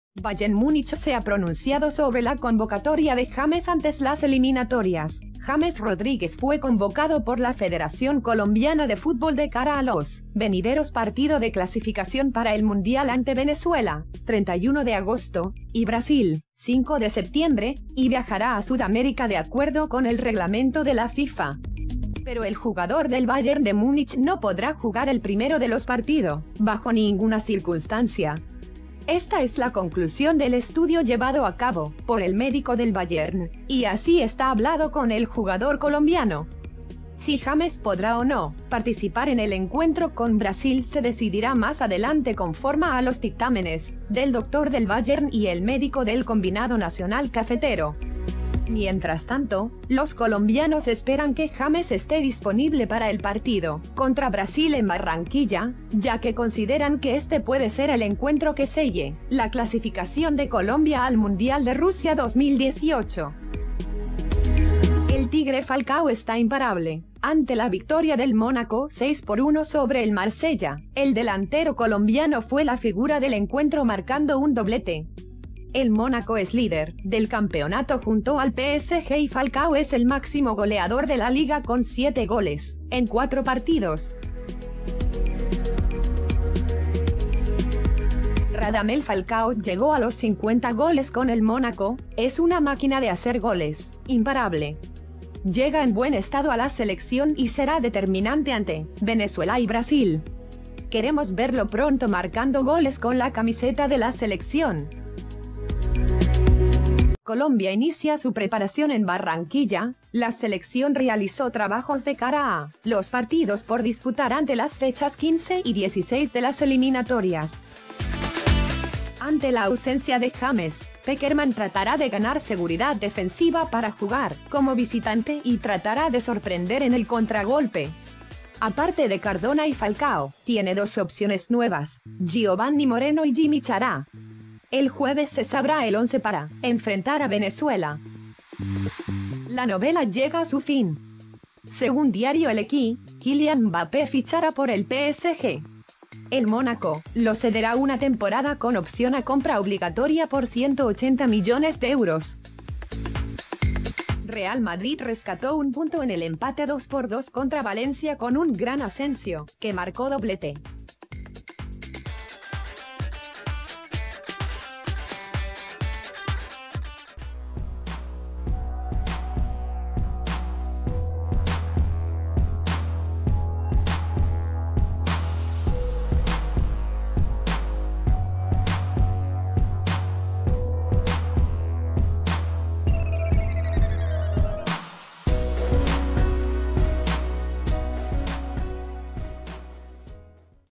DOBLETE DEL TIGRE FALCAO, IMPARABLE! | EL BAYERN PROHÍBE a JAMES JUGAR ANTE VENEZUELA?
FALCAO ESTÁ IMPARABLE, MÁXIMO GOLEADOR De LA LIGA 1...

SE CONFIRMA LA AUSENCIA De JAMES!.

LA SELECCIÓN COLOMBIA SE PREPARA EN BARRANQUILLA...

MÁS.